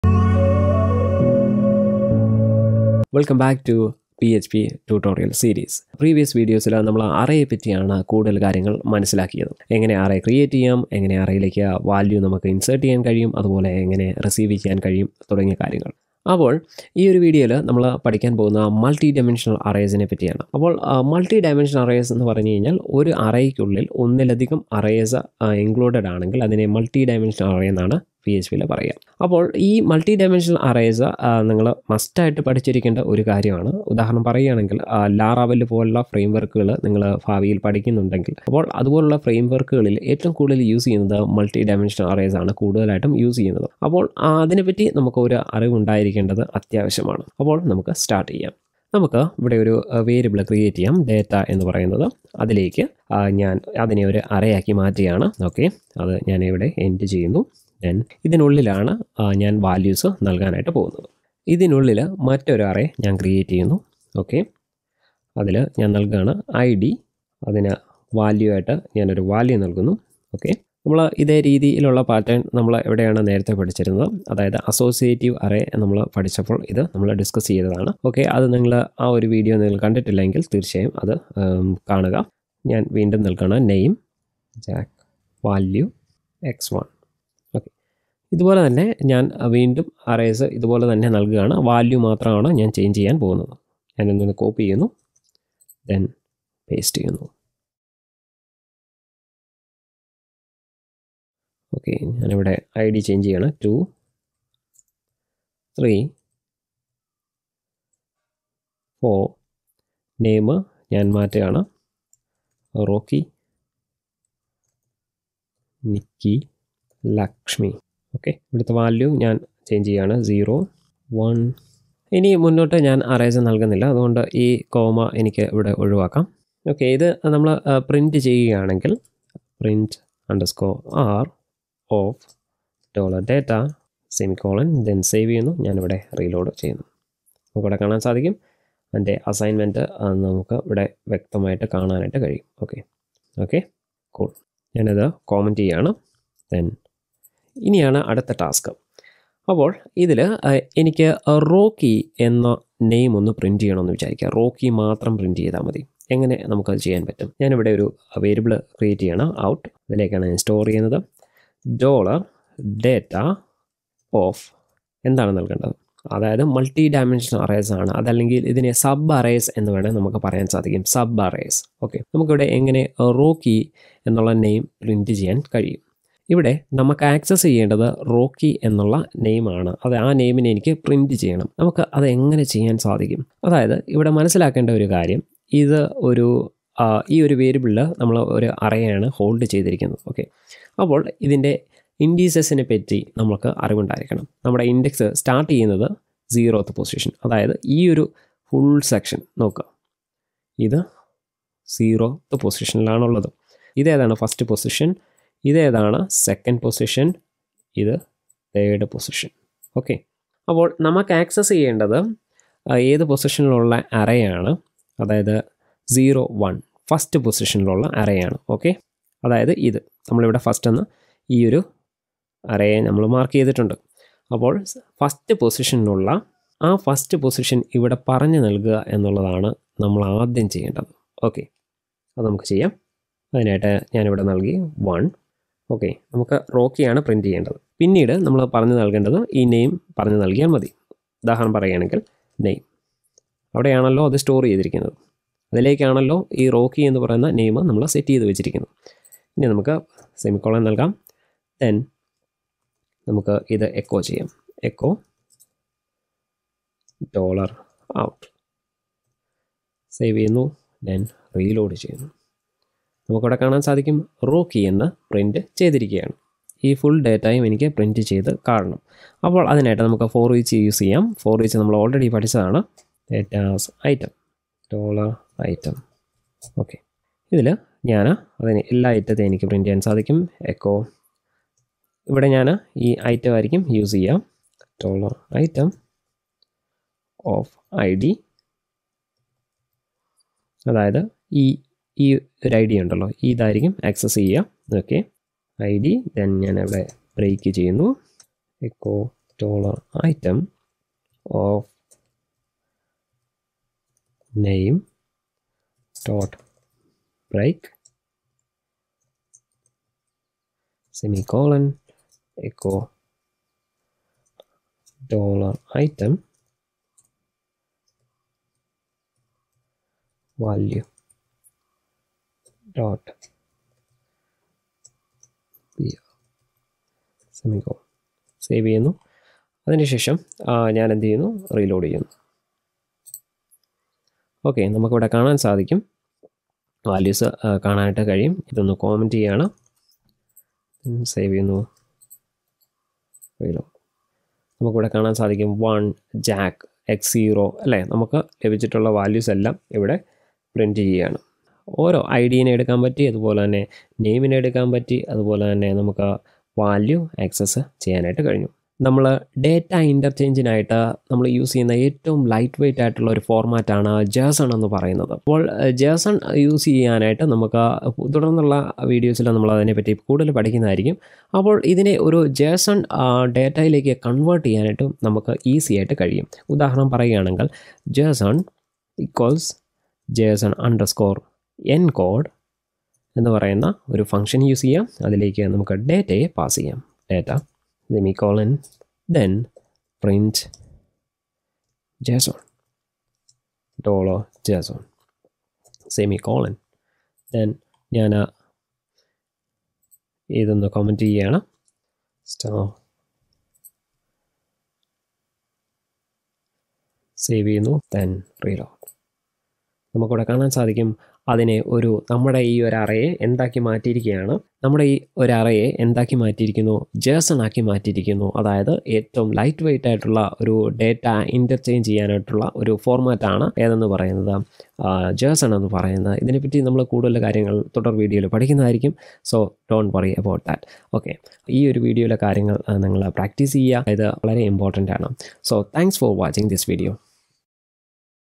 PHP Sub array and JSON Encode | PHP Malayalam Tutorial | Part 15
In this video, we will look into the use of multi-dimensional arrays and PHP's JSON Encode. Let's get started.